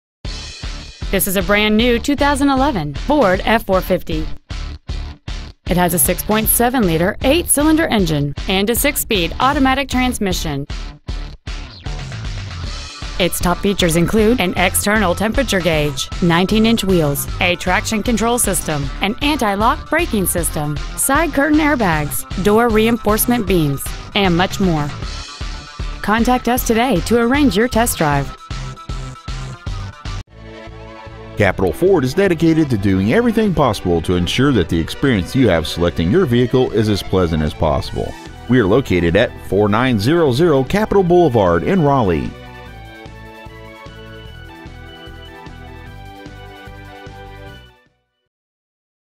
2011 FORD F-450 CHASSIS NC
We are proud to present this 2011 FORD F-450 CHASSIS .

 Year : 2011

 Make : FORD

 Model : F-450 CHASSIS

 Engine : 6.7L V8

 Trans . : AUTO 6SPD

 Exterior : OXFORD WHITE

 Miles : 3

 Interior : STEEL

 4900 Capital Blvd.

 2011 FORD F-450 CHASSIS  NC